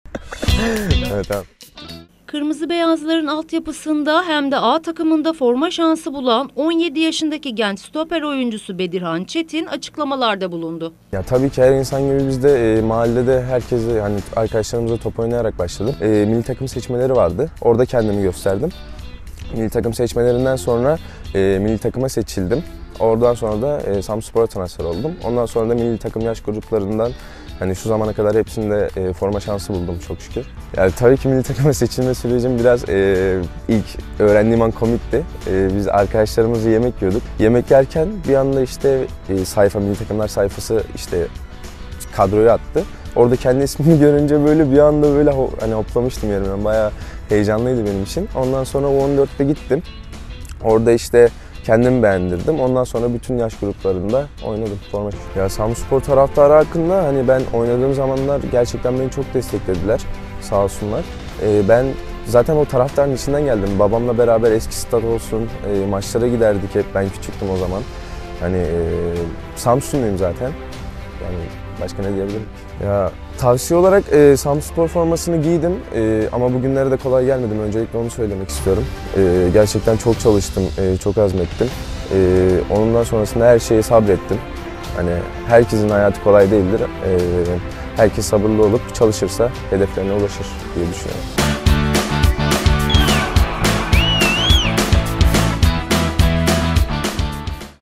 Samsunspor'un genç sporcusu Bedirhan Çetin'den mesaj: "Bugünlere kolay gelmedim"
Samsun Haber

Yılport Samsunspor'un genç oyuncusu Bedirhan Çetin, taraftarın içinden geldiğini belirterek, "Samsunspor formasını giydim. Ama bugünlere de kolay gelmedim. Gerçekten çok çalıştım ve her şeye sabrettim" dedi.